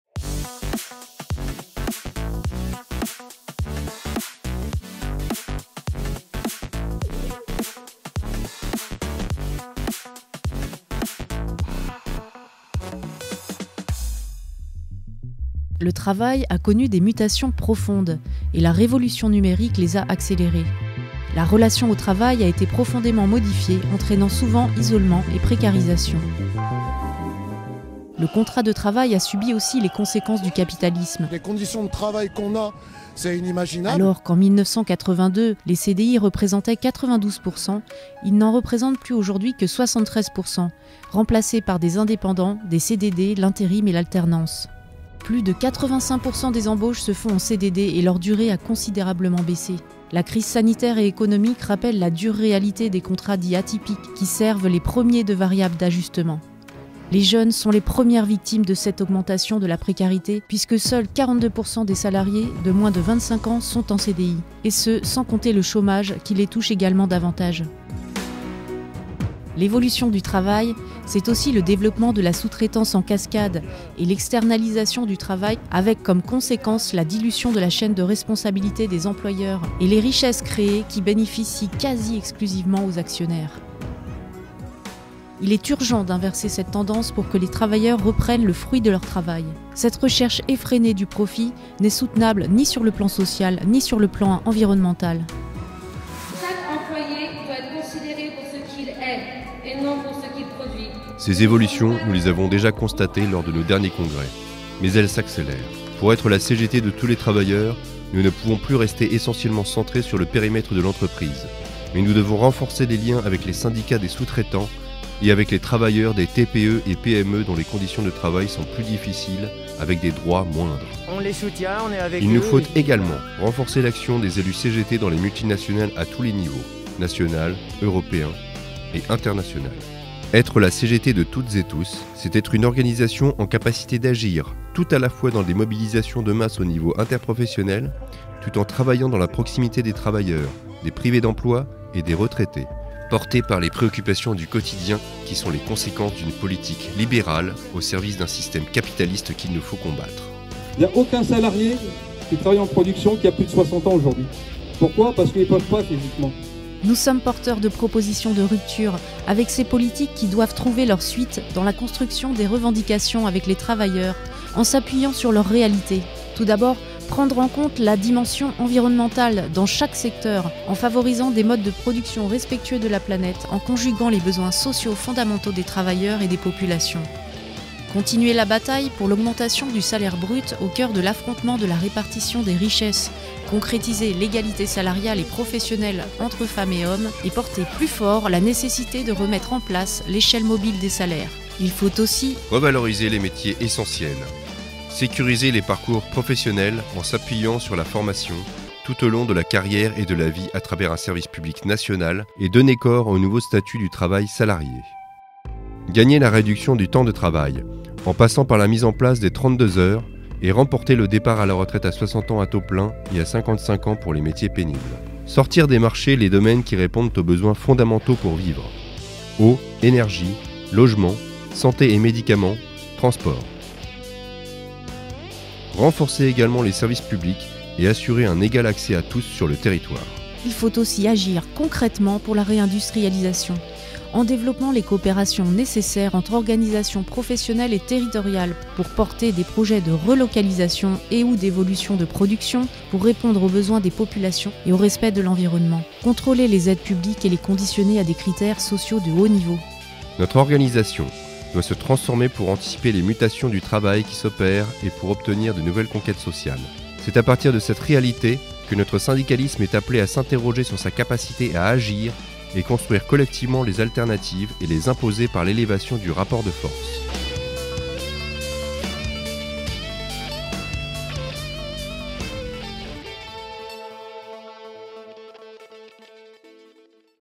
📑 Document d’orientation | Thème 1 : Le syndicalisme au quotidien pour de nouveaux droits
Pour un syndicalisme de rupture et de transformation sociale dans un monde du travail en perpétuelle évolution